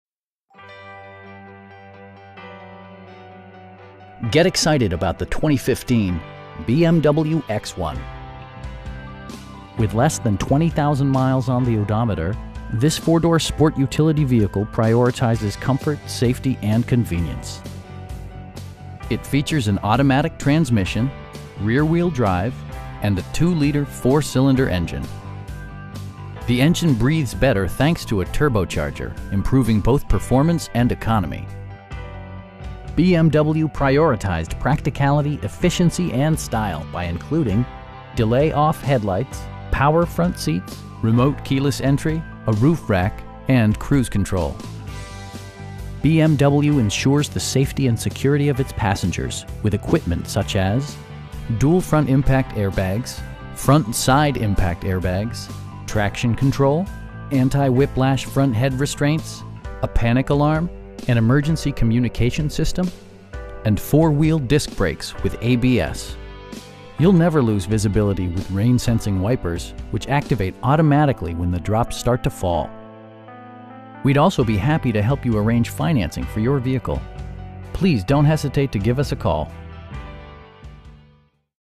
2015 BMW X1 sDrive28i in Charleston, SC 29407
Get excited about the 2015 BMW X1. With less than 20,000 miles on the odometer, this 4 door sport utility vehicle prioritizes comfort, safety and convenience. It features an automatic transmission, rear-wheel drive, and a 2 liter 4 cylinder engine. The engine breathes better thanks to a turbocharger, improving both performance and economy. BMW prioritized practicality, efficiency, and style by including: delay-off headlights, power front seats, air conditioning, tilt and telescoping steering wheel, remote keyless entry, a roof rack, and cruise control. BMW ensures the safety and security of its passengers with equipment such as: dual front impact airbags, front side impact airbags, traction control, brake assist, anti-whiplash front head restraints, a panic alarm, an emergency communication system, and 4 wheel disc brakes with ABS. You'll never lose visibility with rain sensing wipers, which activate automatically when the drops start to fall. We'd also be happy to help you arrange financing for your vehicle. Please don't hesitate to give us a call.